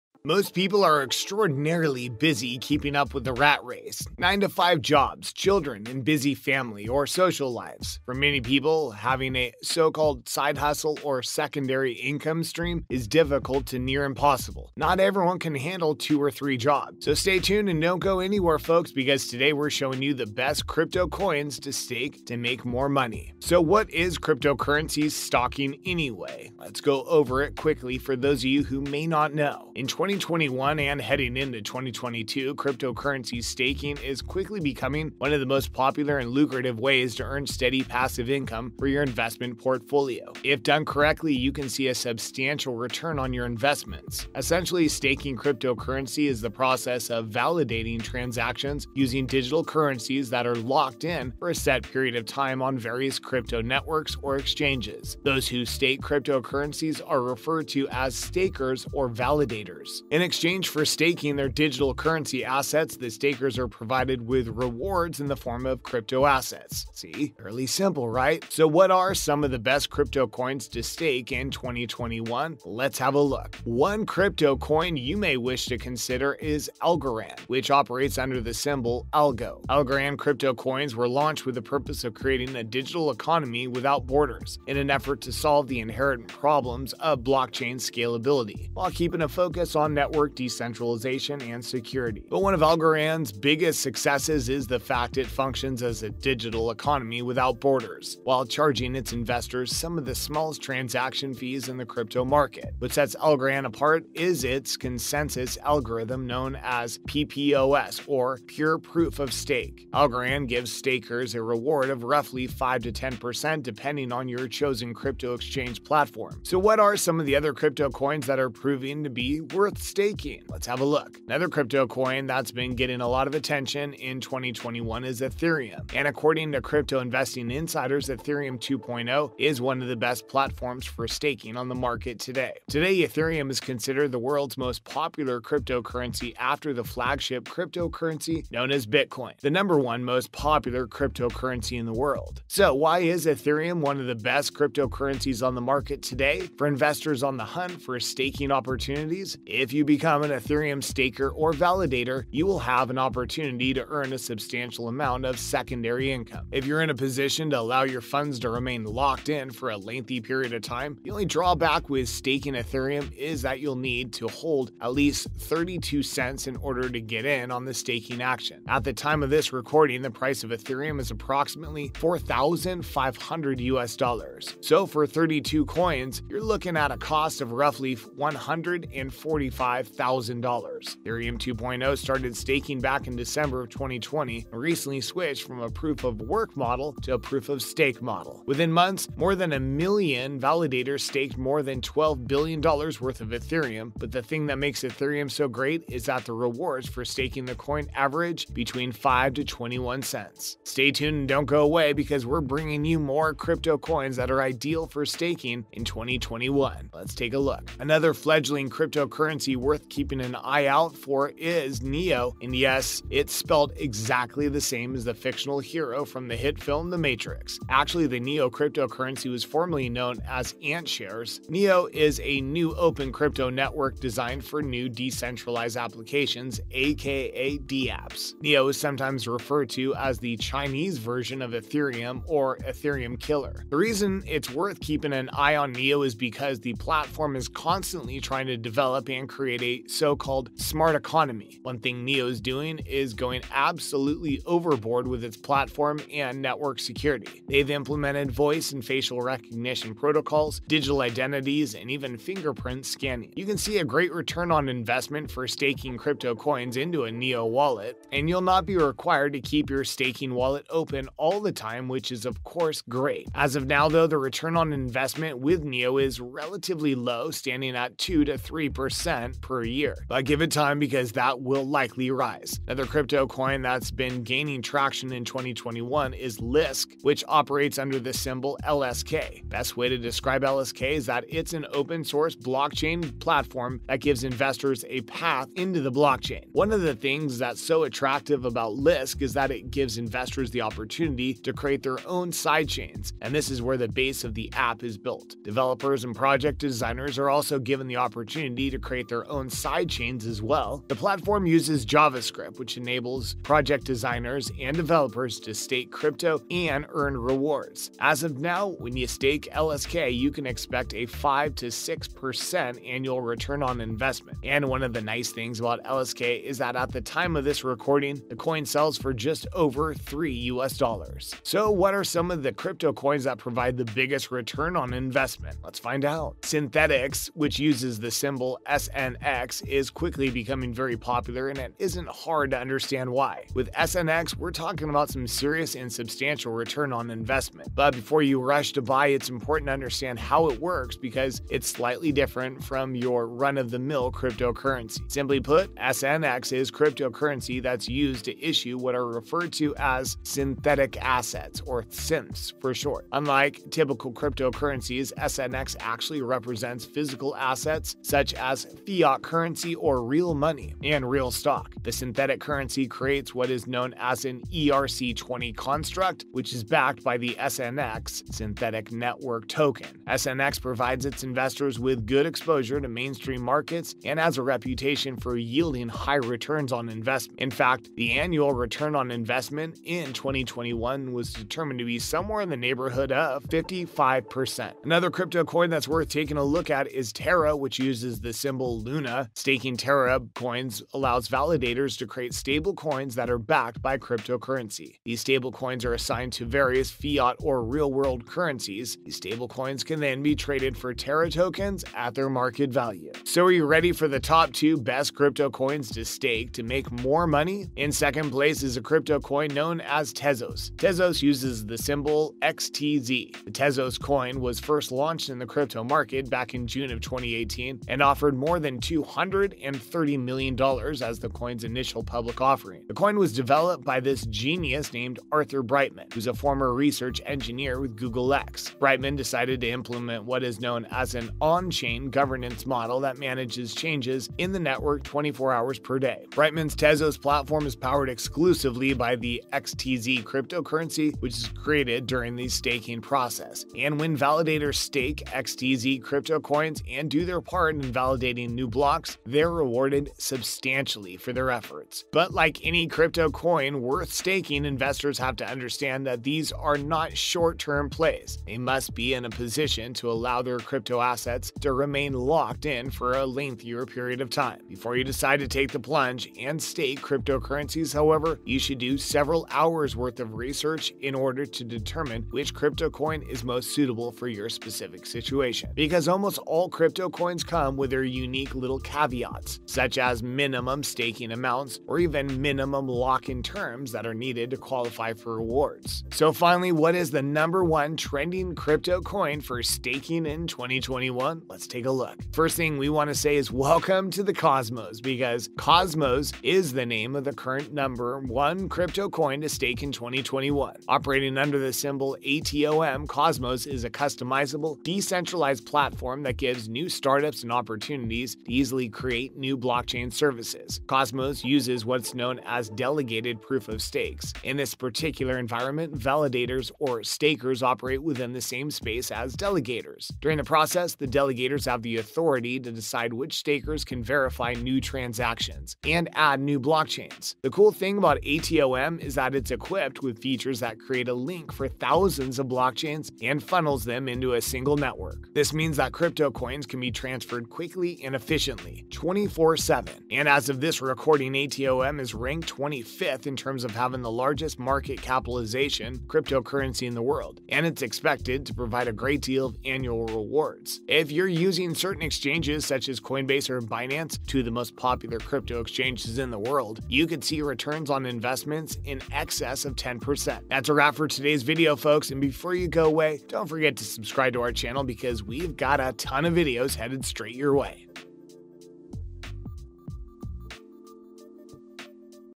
Crypto Coins That You NEED To Stake To Make MASSIVE Money!
Welcome back to Finance Frenzy, today on the channel we are going to find out the Best crypto coins to stake to make more money. From Bitcoin and Ethereum to Dogecoin and Tether, there are thousands of different cryptocurrencies, which can make it overwhelming when you’re first getting started in the world of crypto. To help you get your bearings, these are the top 10 cryptocurrencies based on their market capitalization, or the total value of all of the coins currently in circulation.

Bitcoin (BTC) Created in 2009 by someone under the pseudonym Satoshi Nakamoto, Bitcoin (BTC) is the original cryptocurrency. As with most cryptocurrencies, BTC runs on a blockchain, or a ledger logging transactions distributed across a network of thousands of computers. Because additions to the distributed ledgers must be verified by solving a cryptographic puzzle, a process called proof of work, Bitcoin is kept secure and safe from fraudsters. Bitcoin’s price has skyrocketed as it’s become a household name. Five years ago, you could buy Bitcoin for about $500. As of Oct. 29, 2021, a single Bitcoin’s price was over $62,000. That’s growth of about 12,300%. Ethereum (ETH) Both a cryptocurrency and a blockchain platform, Ethereum is a favorite of program developers because of its potential applications, like so-called smart contracts that automatically execute when conditions are met and non-fungible tokens (NFTs). Ethereum has also experienced tremendous growth. In just over five years, its price went from about $11 to over $4,400, increasingly nearly 40,000%. Watch the entire video and learn the Best crypto coins to stake to make more money.